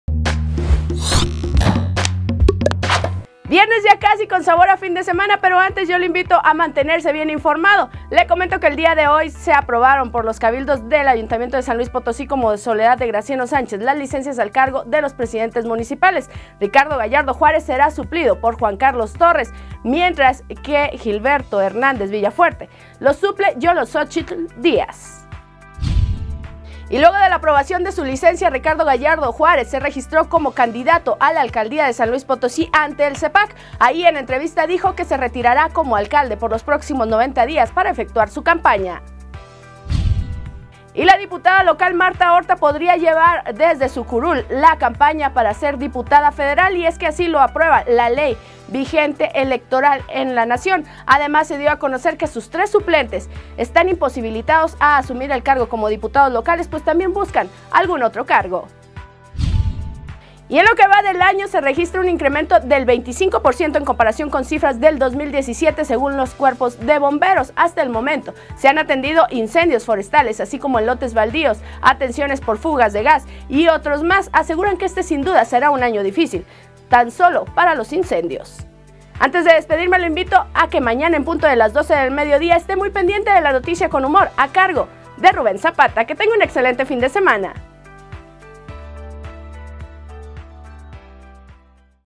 Resumen Informativo del día 23 de marzo del 2018
Resumen Informativo del día 23 de marzo del 2018 con Diana Rangel​.
-Aprueba Cabildo municipal licencia del alcalde Ricardo Gallardo Juárez/Alcalde de Soledad pidió licencia a su cargo
-Ricardo Gallardo Juárez se registró como candidato a la presidencia ante el Ceepac
-Martha Ortha podría hacer campaña para diputada federal sin pedir licencia a su cargo
-Incendios ya superan en 25% siniestros del año pasado; se contabilizan mil 500